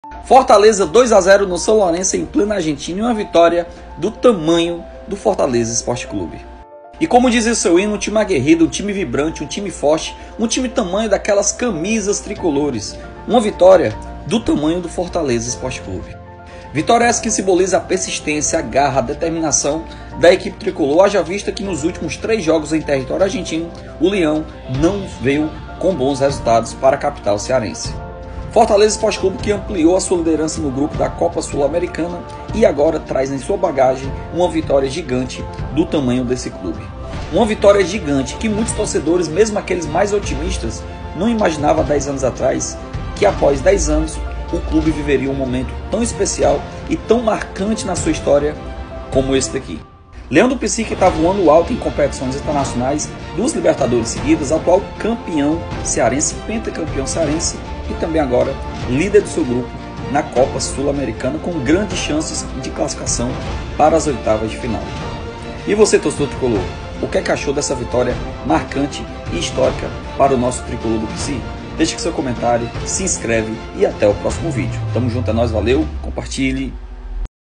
Vitória Histórica - Fortaleza 2 x 0 SAN Lorenzo em plena Argentina.
notícias do Fortaleza ec
notícias do Fortaleza Esporte Clube

Libertadores,Copa libertadores 2023, Fortaleza gols libertadores,Notícias do Fortaleza,Salários dos jogadores.